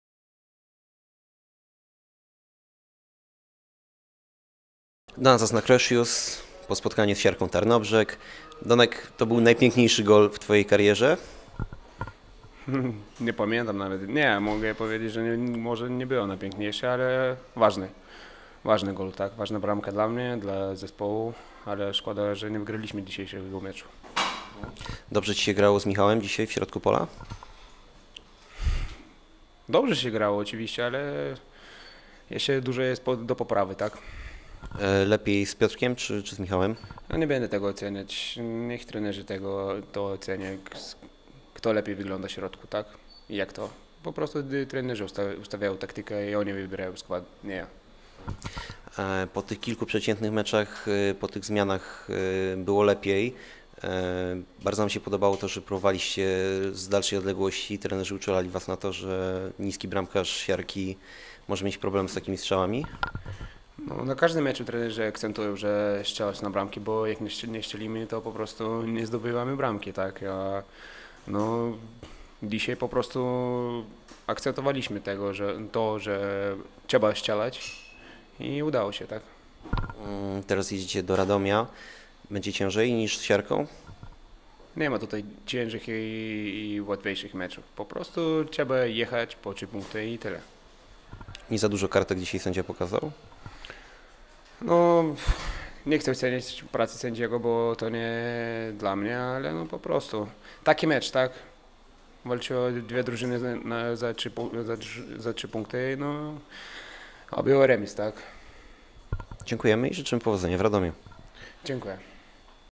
Donatas Nakrošius po meczu Polonia - Siarka Tarnobrzeg
Strzelec pierwszej bramki dla Polonii o dzisiejszych zawodach...